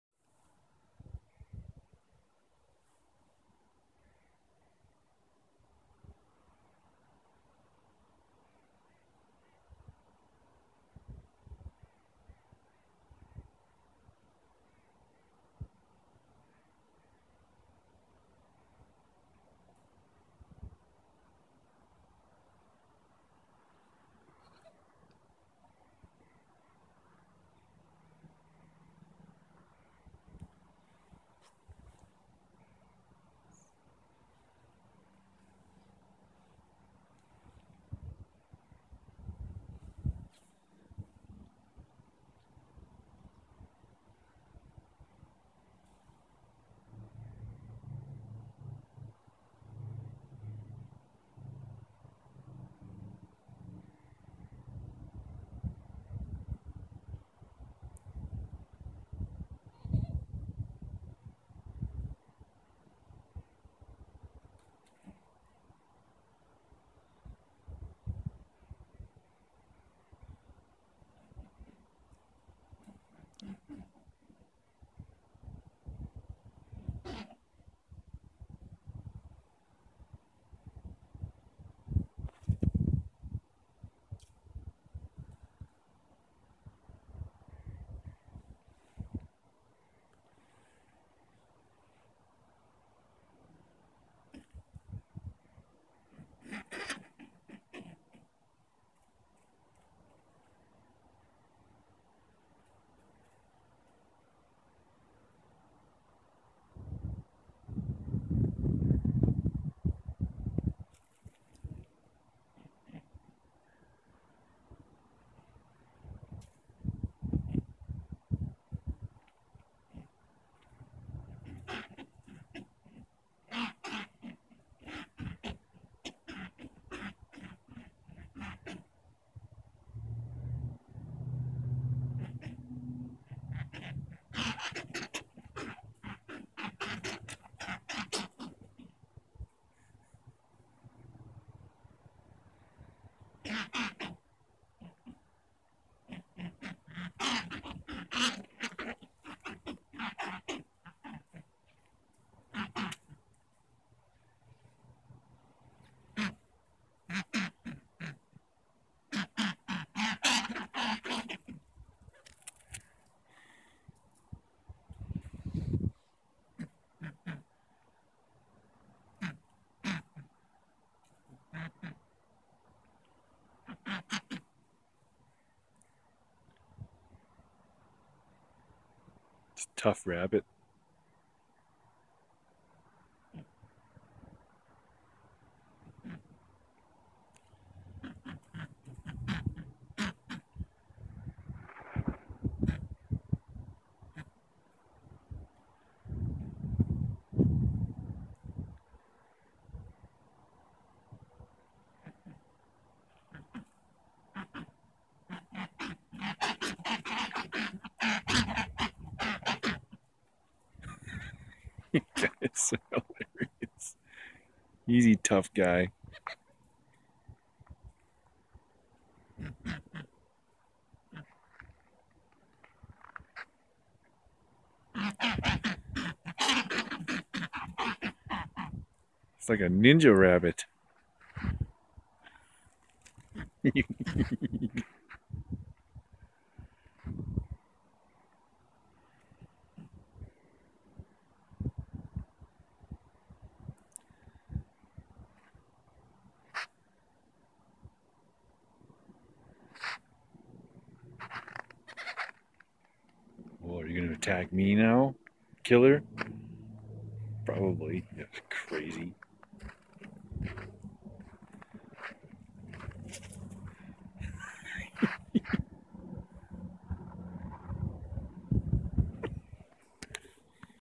Fighting Rabbits
Caught these three having a turf war in the back lane. Never seen an aggressive bunny rabbit outside of a Monty Python movie. (Didn't notice if these ones had fangs but they sound pretty awesome). Kind of long but worth the wait to see them throw down. Skip to 2:00 if you don't want to go through all the building tension... :)